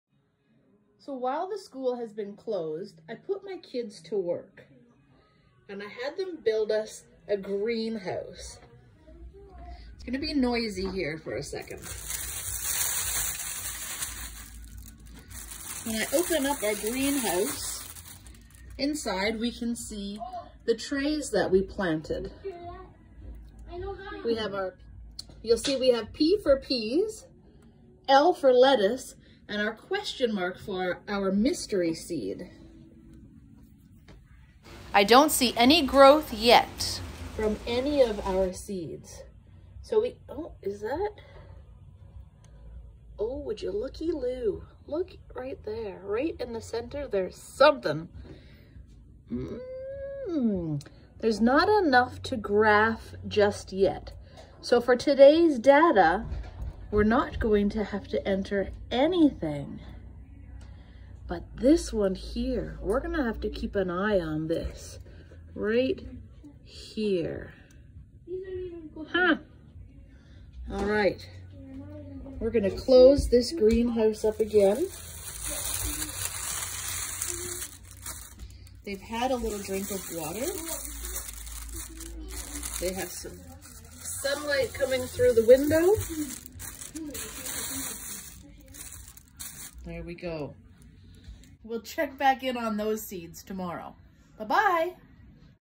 Thursday, April 23, 2020 - Checking the seeds - Video #2 for today
Yesterday we planted our seeds.  Today we need to check in on them to see if there's anything to graph!!  If they haven't started to grow we will leave our graph blank.  Just wait until you see what my kids made for us!!  They've been busy!!